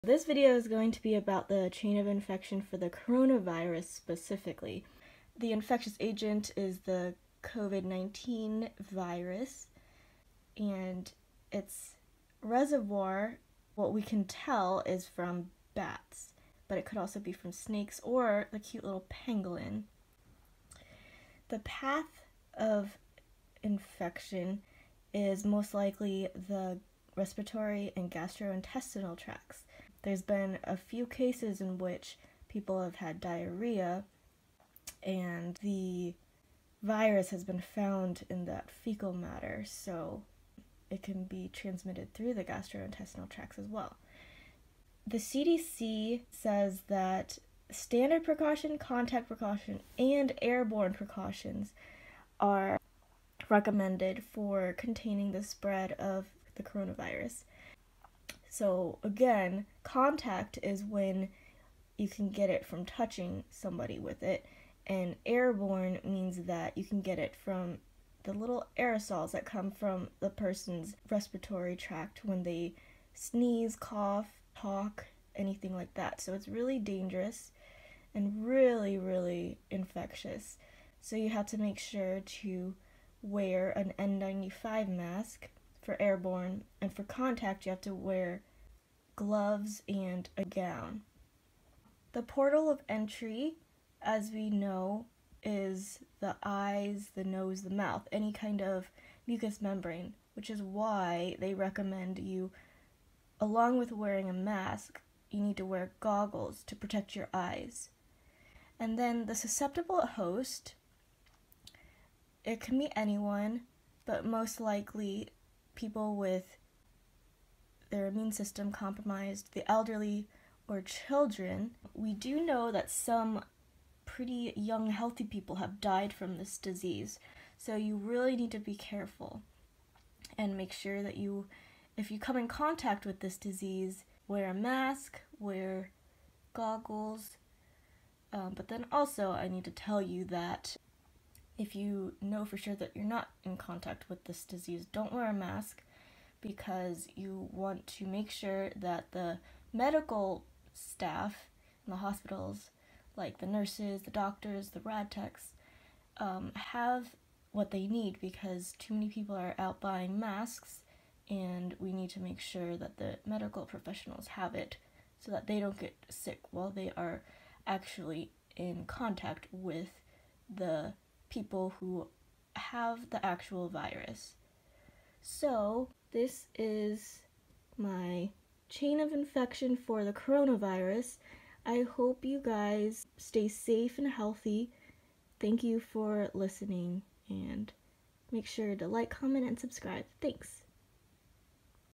Chain of Infection for the Coronavirus
This video explains how the coronavirus is spread with the chain of infection.